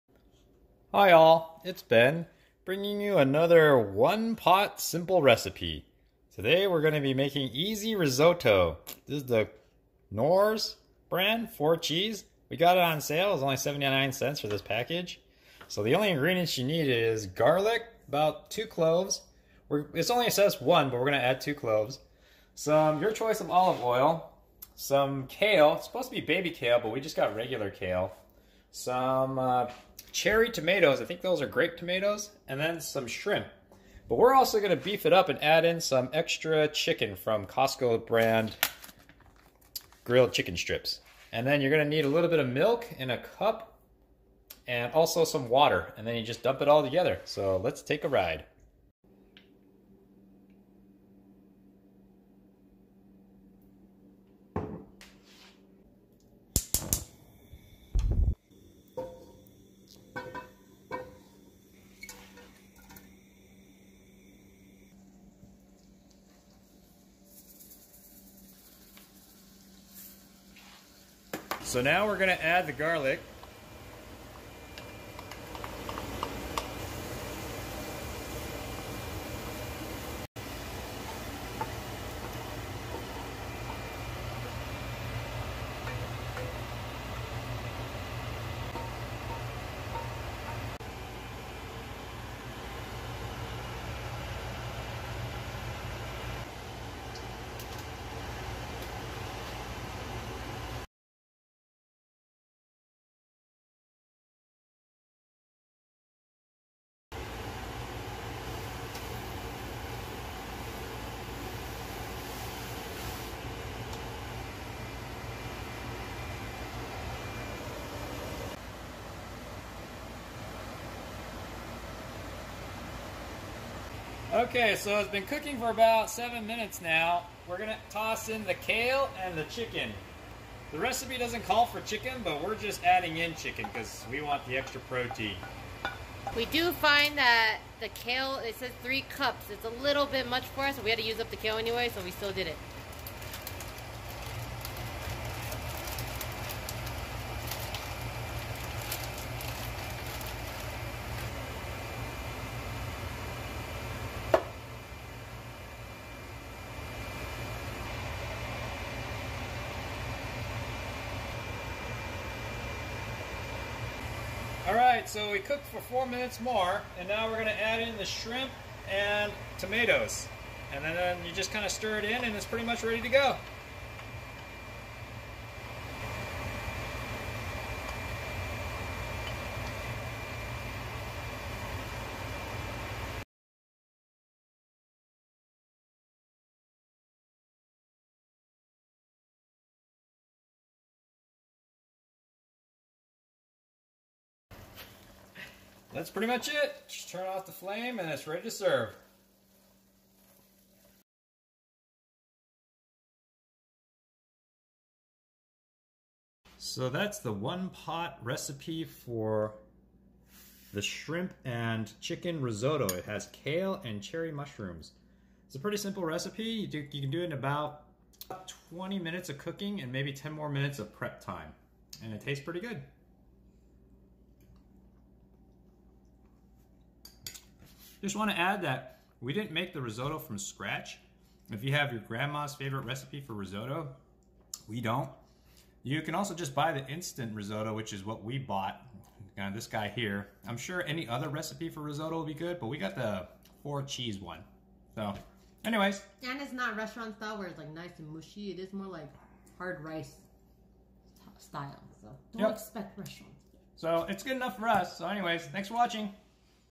DIY - Easy 4 Cheese Risotto with shrimp, kale, chicken and tomatoes (One pan simple recipe)
Here is another straightforward recipe for a 4 cheese risotto. We used Knorr brand instant risotto. If you have your own recipe that's great. The recipe in this video makes about 4-6 servings depending on big you consider your serving size to be. The recipe is also on the back of the Knorr package.

Ingredients
1 package 4 Cheese Easy Risotto (Knorr)
3 cloves of garlic (chopped)
3 cups of kale (chopped)
1 cup cherry or grape tomatoes (cut in half)
3 Tbsp Olive Oil
0.5 - 1 lb shrimp (raw, peeled and deveined)
1 cup milk (we used 2%)
1.5 cup waterr
Optional: Half package (0.75 lbs) precooked grilled chicken

Cooking time is about 15 mins. Preparation time is about 10 mins.

1) Heat 1 Tbsp olive oil in pan on medium heat and add shrimp and cook for about 2 mins 30 seconds. Then add garlic and cook/sautee together with shrimp for 30 seconds more. Afterwards, remove shrimp and garlic and set aside.

2) Pour in milk, water, 2 Tbsp olive oil, and entire packet of risotto mix into same pan. Bring to boil then turn heat to medium and cook for 7 mins. Toss in kale and chicken (optional) and stir and cook for 4 more mins.

3) Stir in shrimp/garlic and tomatoes. Once thoroughly mixed, turn off heat and serve.

That's it. It's a pretty simple recipe and all in one pan. Again I'm not a gourmet chef or anything so if you have a better recipe for risotto or the idea of using instant risotto repulses you feel free to make any modifications.